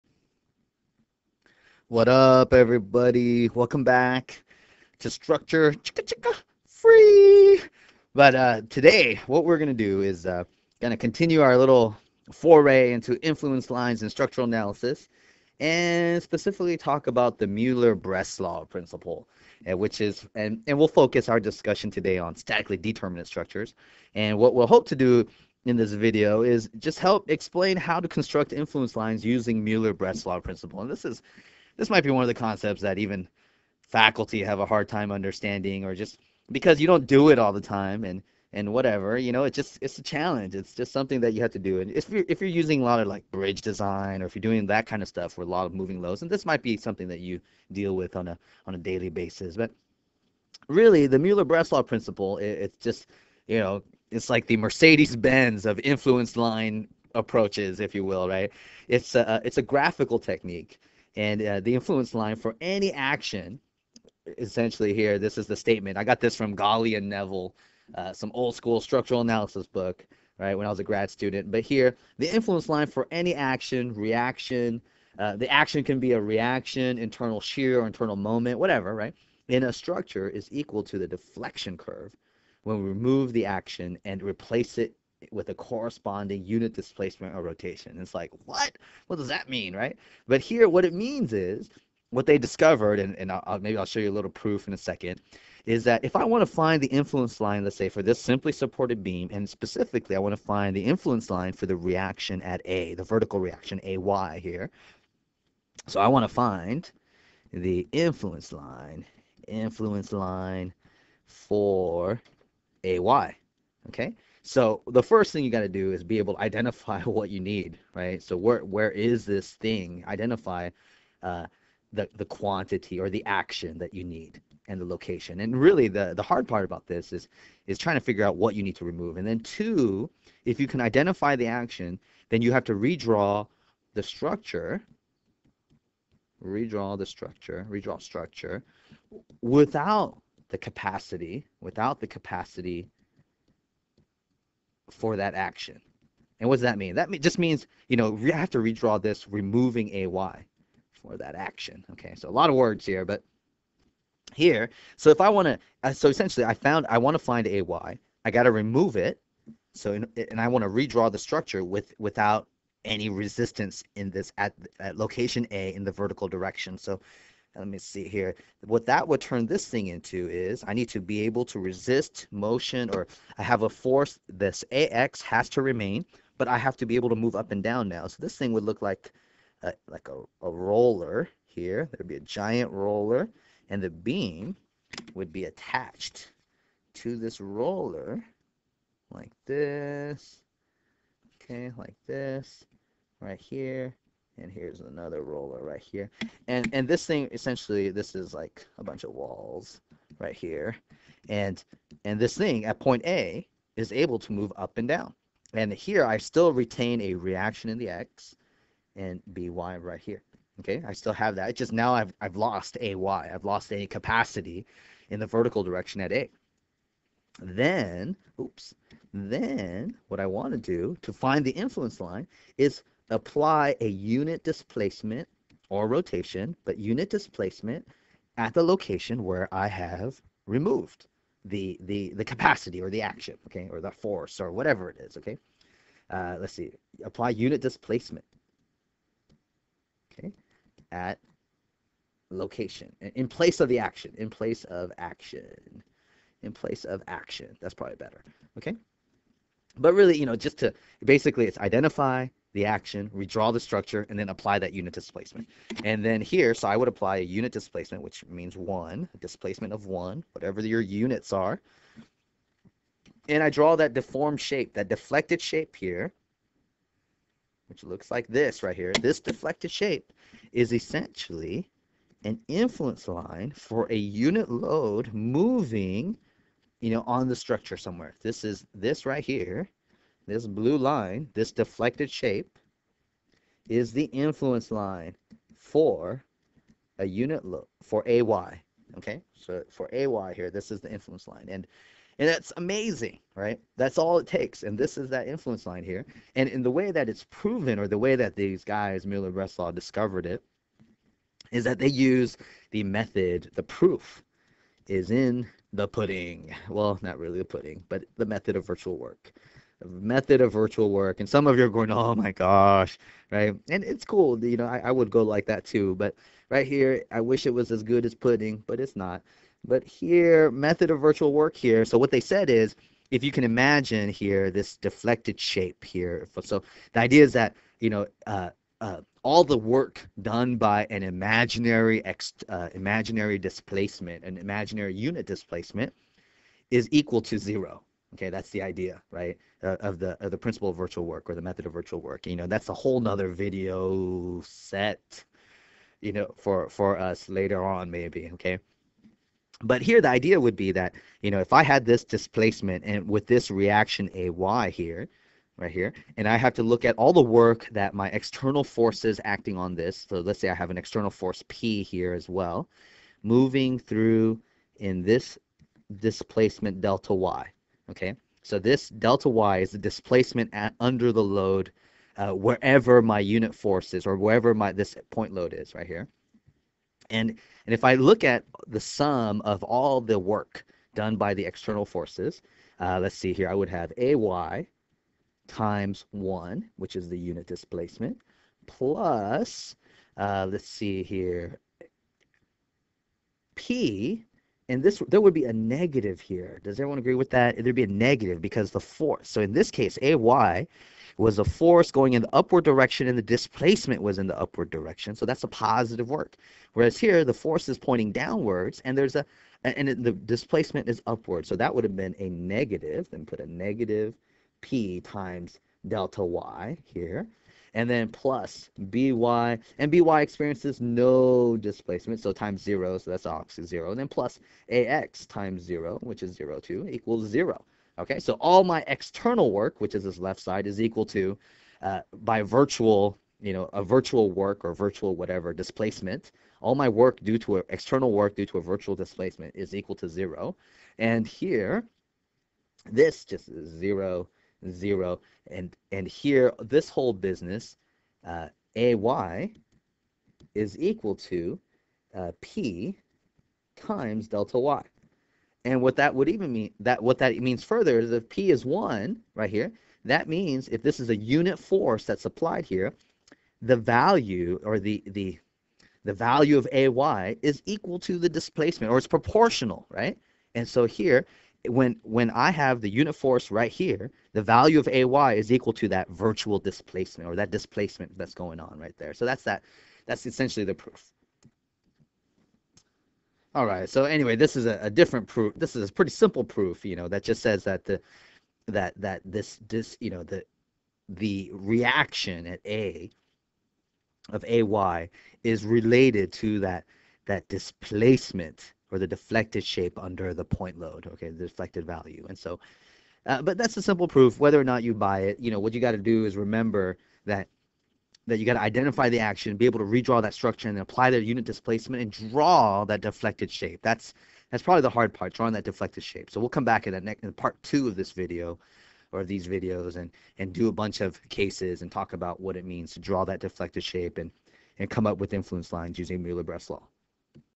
Influence Lines with Muller-Breslau Principle (part 1) - Structural Analysis
Explaining how to use the Muller-Breslau Principle to draw influence lines for statically determinate beams.